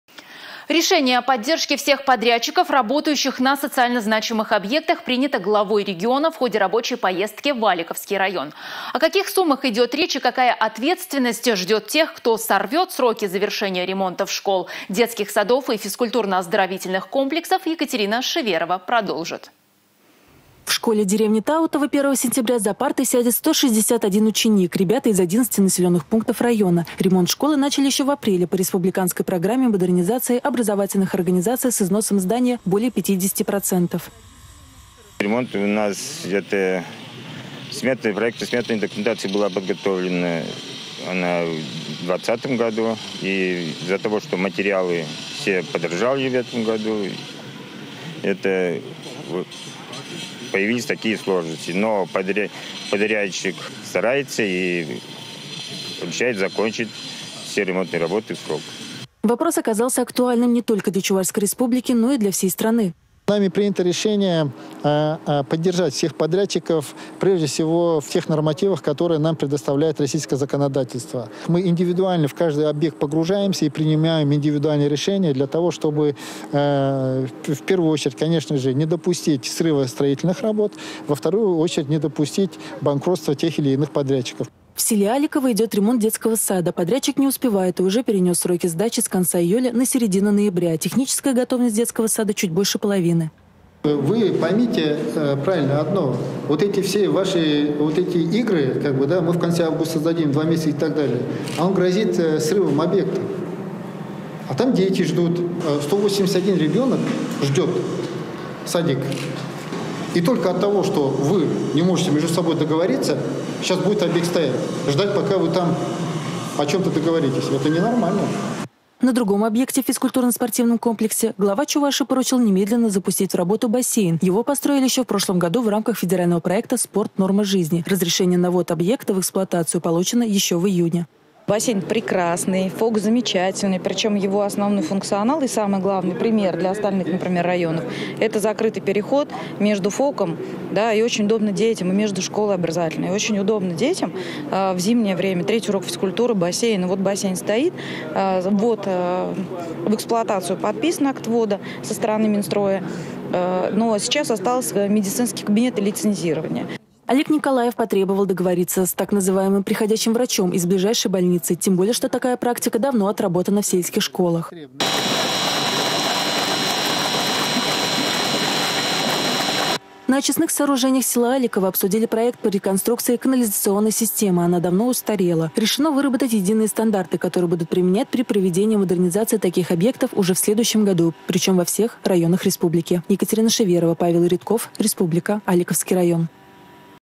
Подрядчикам, работающим на социально значимых объектах окажут поддержку
Решение о поддержке всех подрядчиков, работающих на социально значимых объектах принято Главой региона в ходе рабочей поездки в Аликовский район. О каких суммах идет речь и какая ответственность ждет тех, кто сорвет сроки завершения ремонтов школ, детских садов и физкультурно-оздоровительных комплексов. Екатерина Шиверова продолжит.